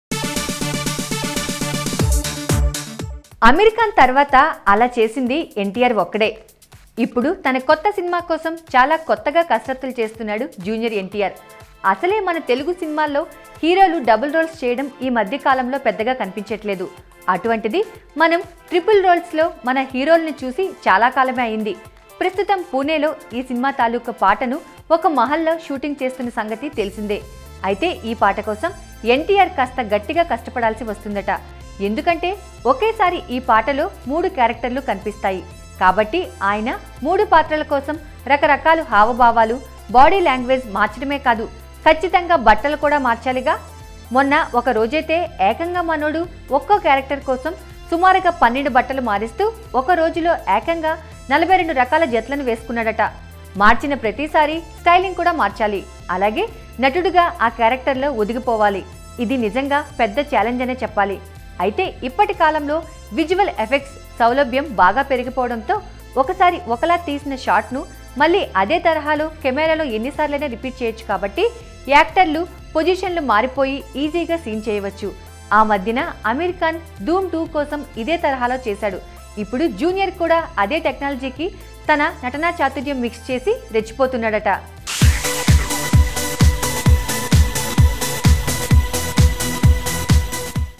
వామ్మో రోజుకి 42జతలా.., | Ntr Amazing Costumes in Jai Lava Kusa | YOYO Cine Talkies
Ntr Ultimate Performance In Jai Lava Kusa.
వామ్మో రోజుకి  42జతలా.., అమీర్ ఖాన్ తర్వాత ఆలా చేసింది ఎన్టీఆర్ ఒక్కడే

Jai Lava Kusa
Film
Jai Lava Kusa is an upcoming Telugu language action-drama film written and directed by K. S. Ravindra. The movie features Jr. NTR, Raashi Khanna and Niveda Thomas in the lead roles.
Director: K. S. Ravindra
Music director: Devi Sri Prasad
Production company: N. T. R. Arts
Cinematography: Chota K. Naidu
Producer: Nandamuri Kalyan Ram